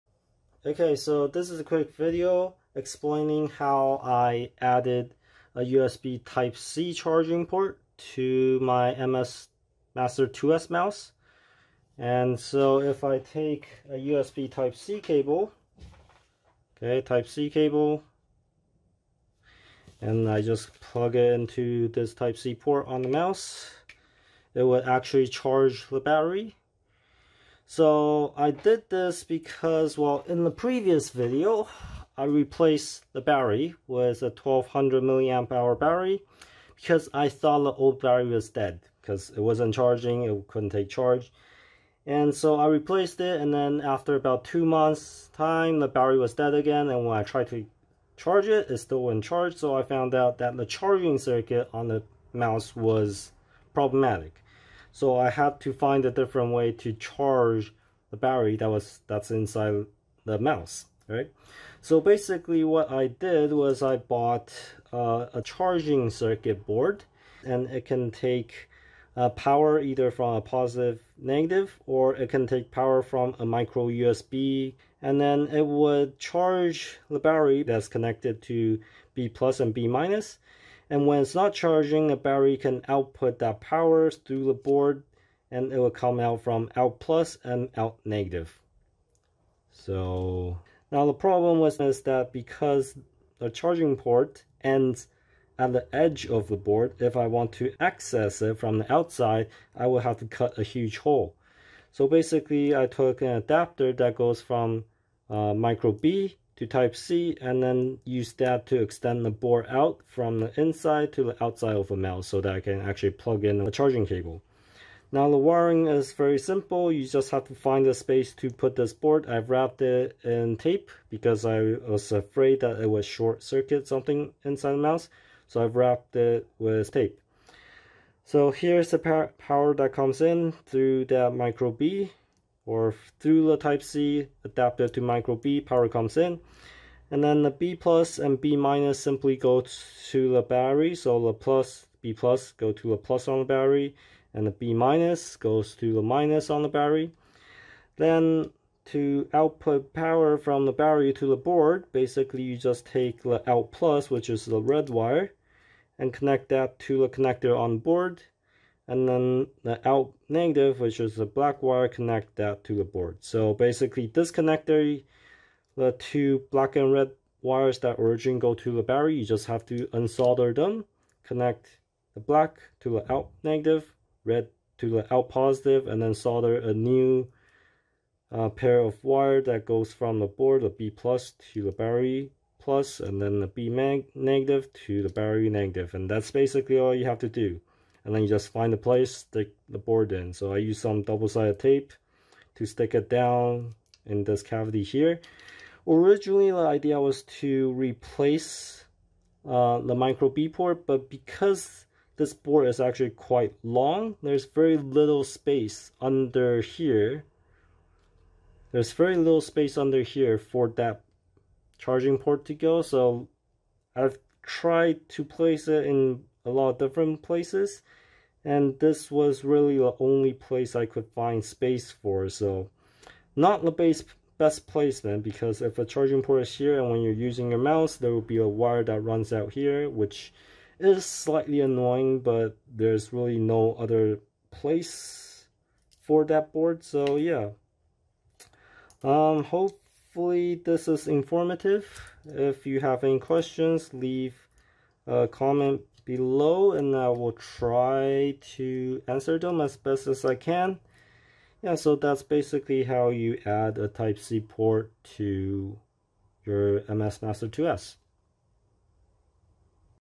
Upgrade MX Master 2S with USB-C [Tutorial]
TP4056 IC + micro B to C adapter.
There are charging boards with type C port, but they're more expensive, and harder to find.